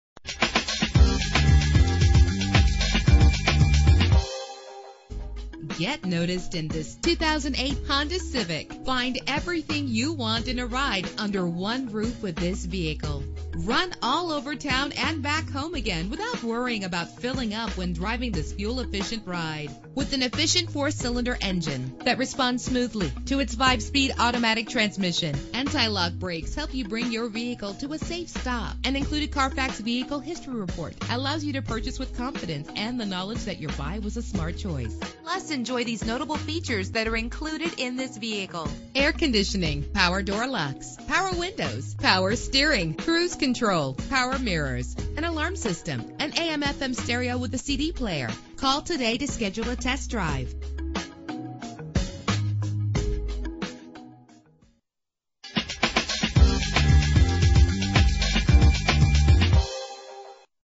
2008 Honda Civic - Wayne NJ
Year: 2008
Make: Honda
Model: Civic
Trim: LX
Engine: 1.8 liter inline 4 cylinder 16 valve
Transmission: 5-Speed Automatic
Color: Blue
Mileage: 32918
Address:
444 Route 46